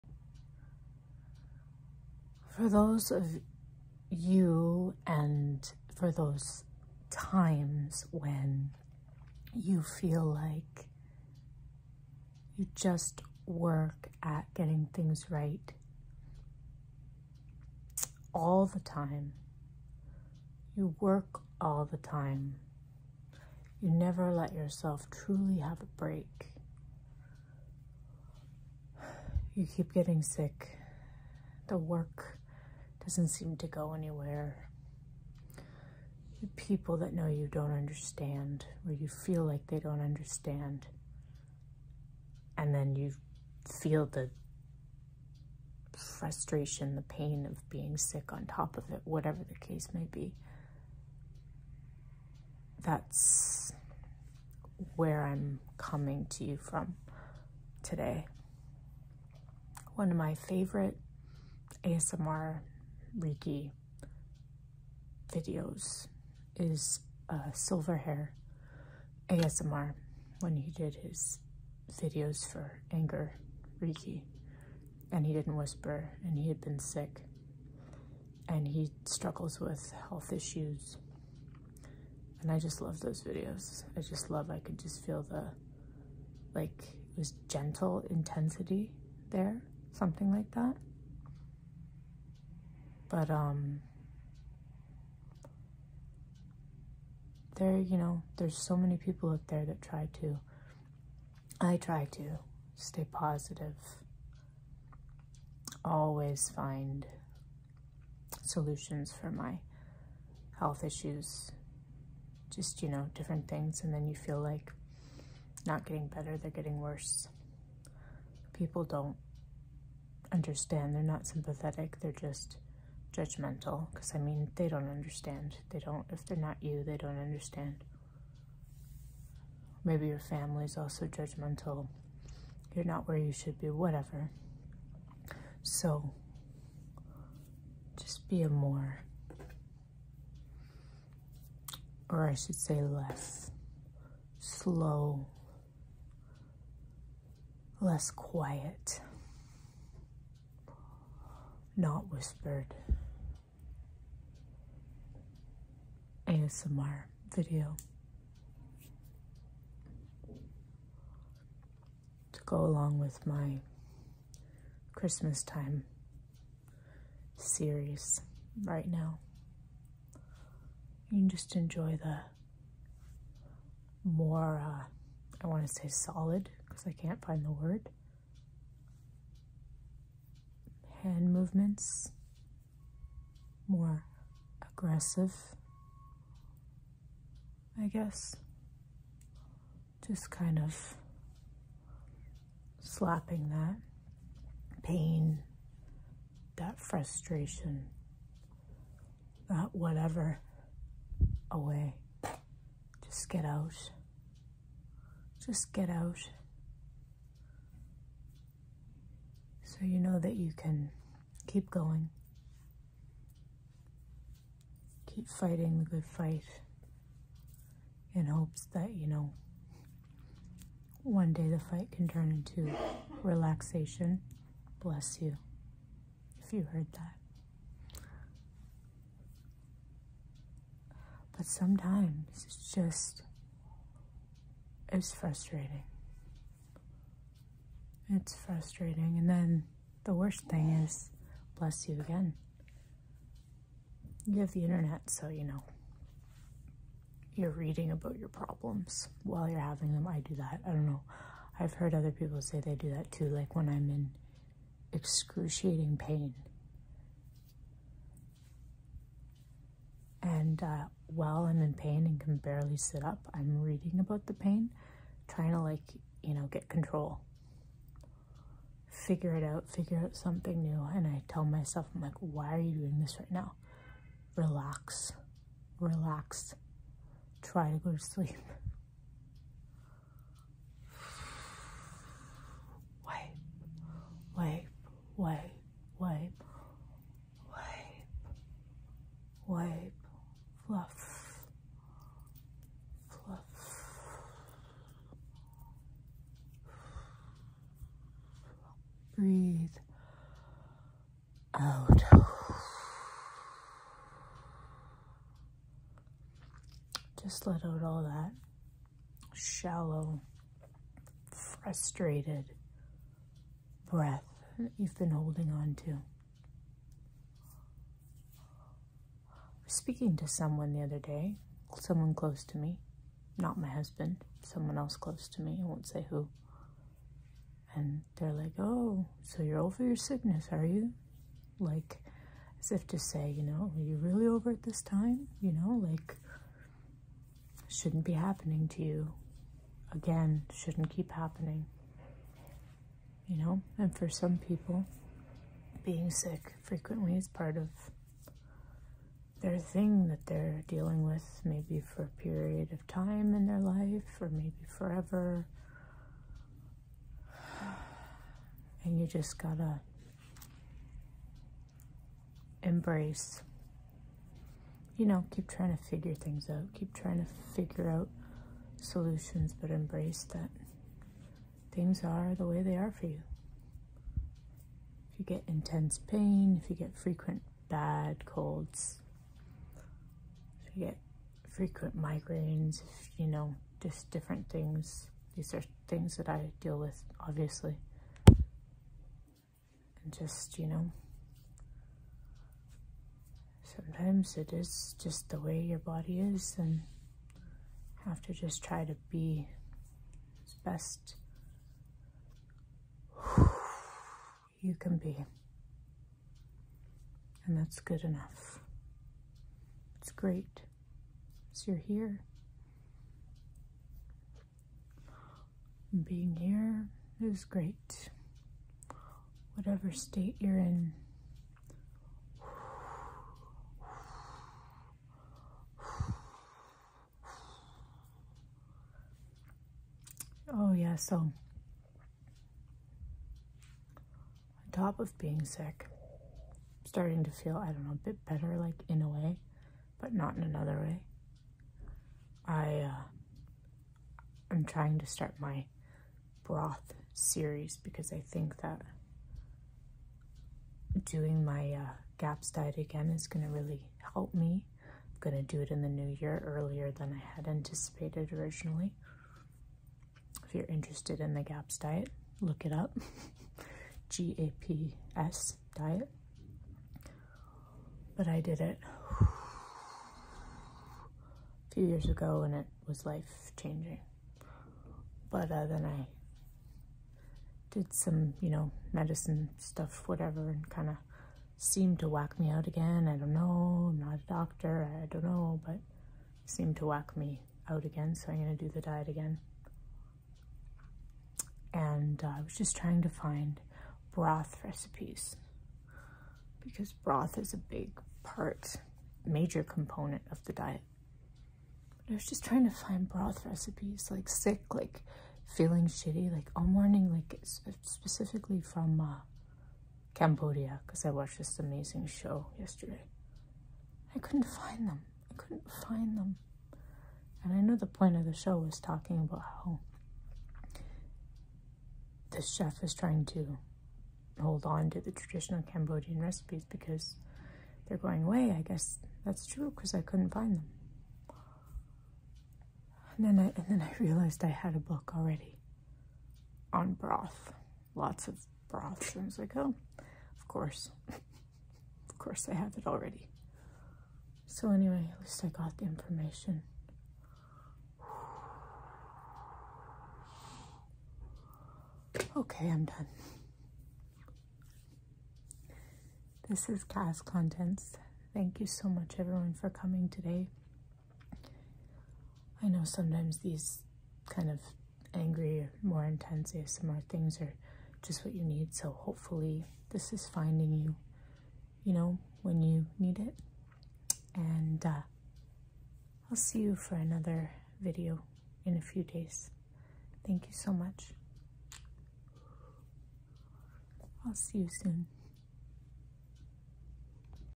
Aggressive ASMR For Anger
Sometimes we're just having a real sh%%ty time and need to let it out somehow.
Sometimes we're tired of being sick despite trying to do everything right, tired of feeling like our work isn't paying off, just tired and frustrated.

So I wanted to make this asmr video today that was a bit faster, maybe more intense, aggressive, if you are just having a time right now.

One of my favorite asmr, actually asmr reiki videos was silverh&&e when he made the video about reiki for when you are expereincing anger. I loved the non-whisper video, I loved the little bit of intensity, it is a great video.

Hopefully this asmr video finds you today and you can feel a bit of relief!!